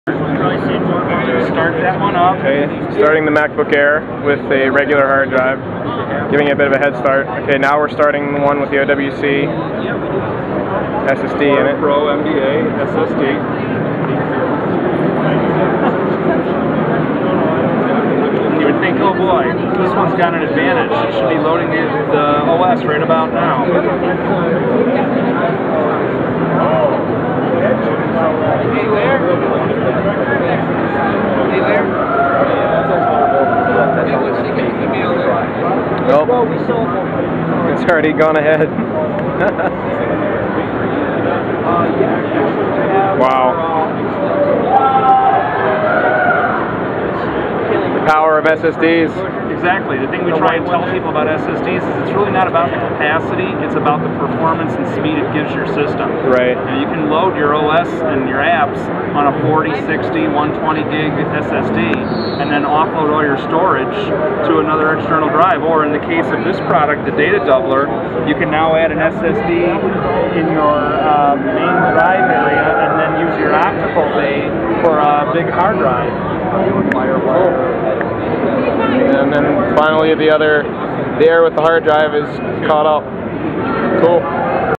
Macbook Air Boot Test @ CES 2011
Macbook Air boot test with one system using the Aura Pro SSD from OWC and the other using a normal SSD.